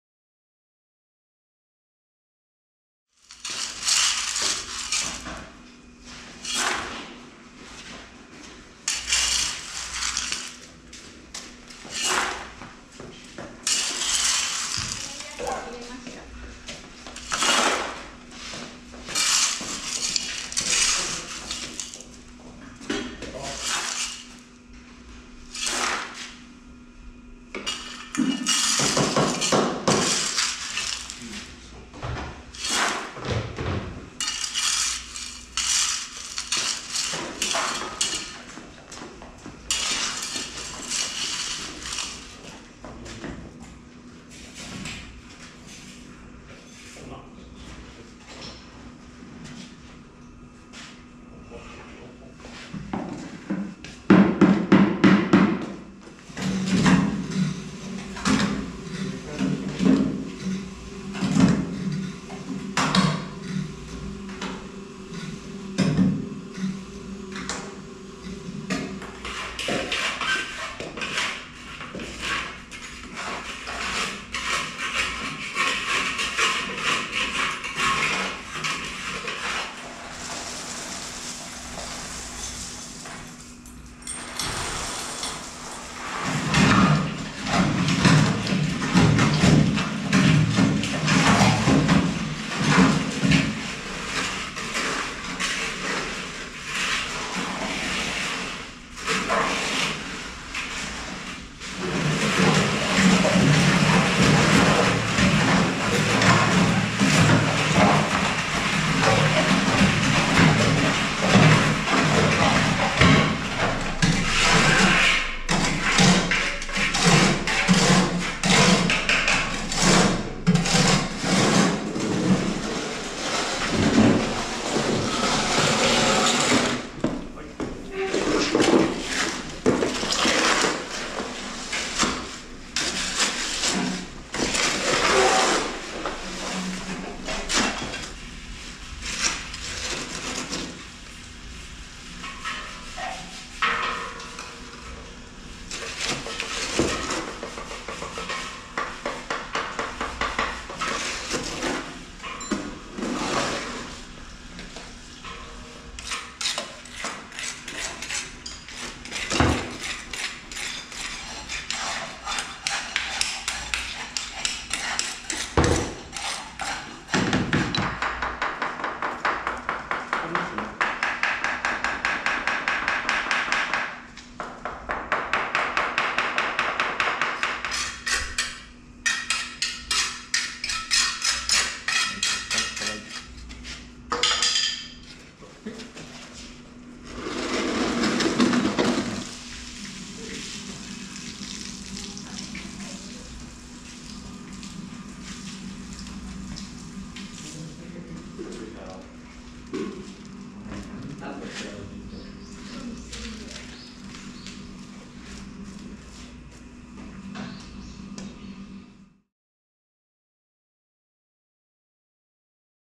＃034 Placing Geopolymer Concrete (Laboratory Test)
We used a 360-degree camera to take movies of the inside of the laboratory.